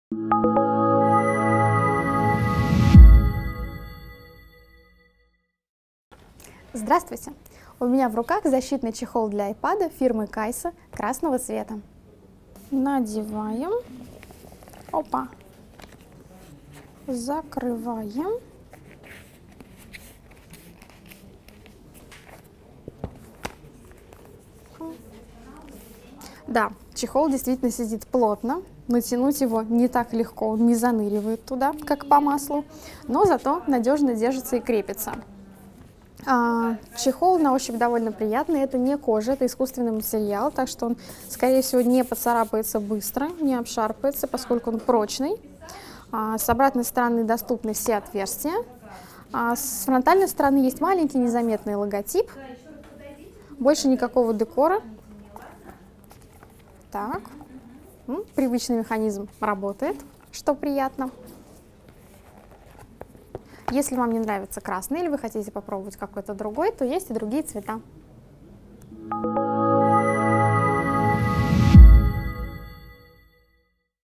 Чехол Kajsa Svelte3 красный
Чехол премиального класса для нового iPad. Стильный и удобный, он отлично защищает iPad от царапин, вмятин, мелких повреждений и пыли. Поддерживает функции автоматического включения и выключения iPad. Кроме функций чехла выполняет роль подставки — и позволяет устанавливать iPad под двумя разными углами наклона.